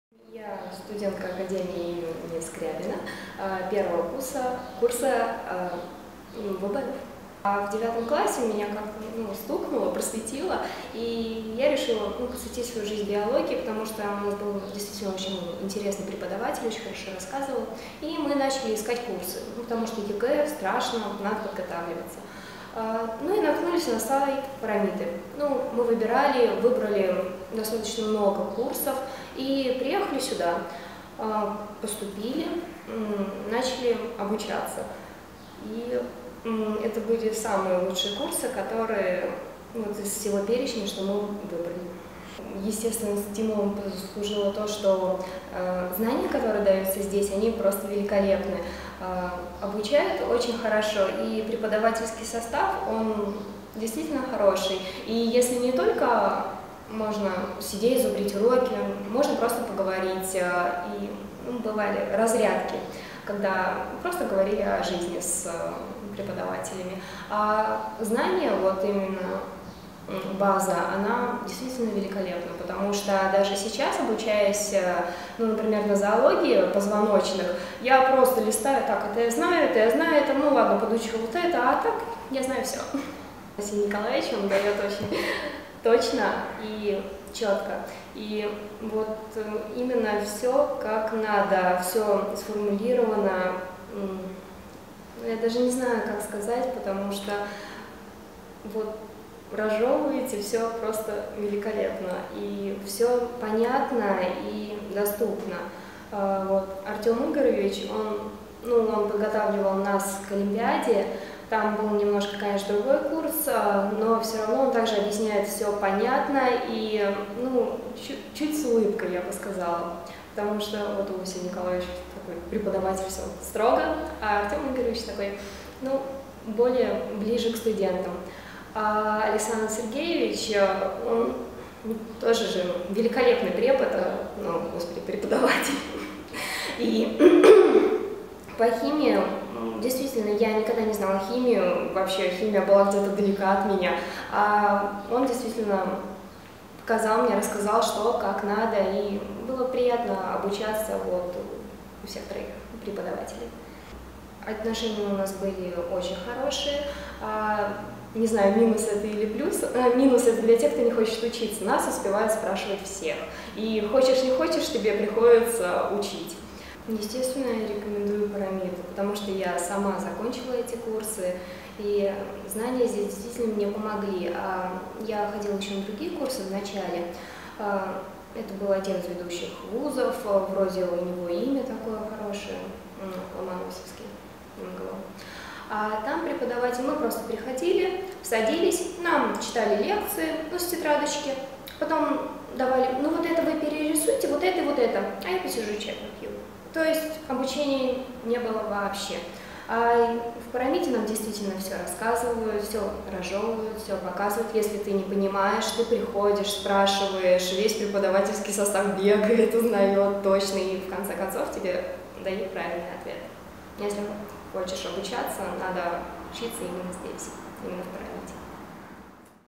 Отзыв Шаталовой Анны
Отзывы учеников об Образовательном ЕГЭ-Центре "Парамита".